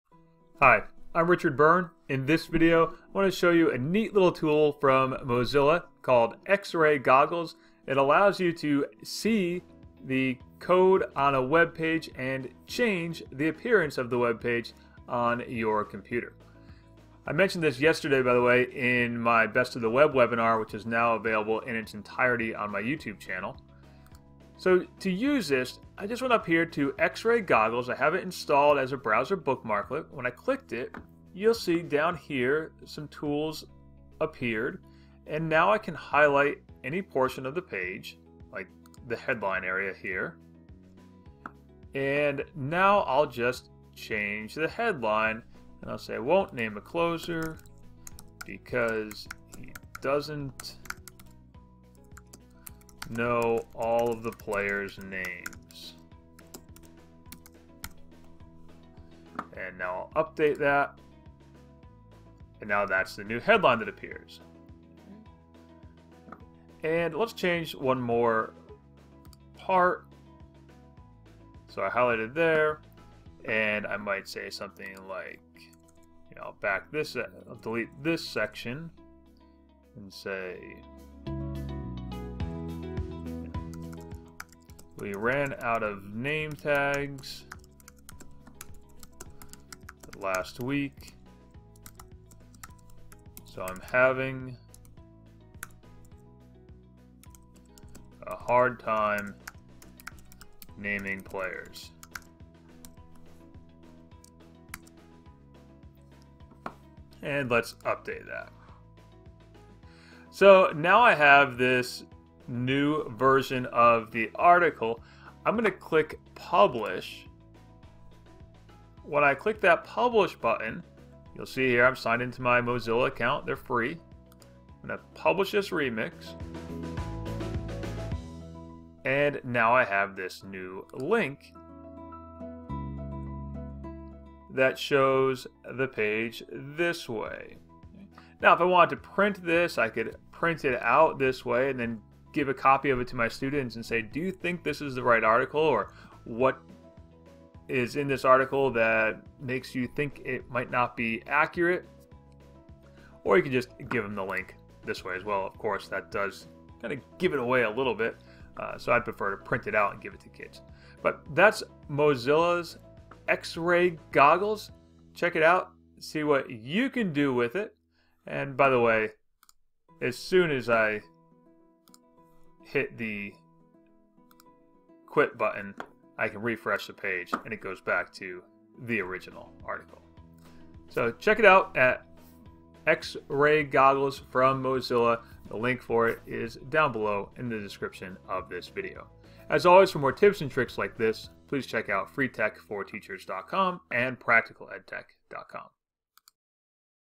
How to Use Mozilla's X-Ray Goggles
Mozilla's X-Ray Goggles let you view the code behind a web page and change that code to make the page look different on your computer.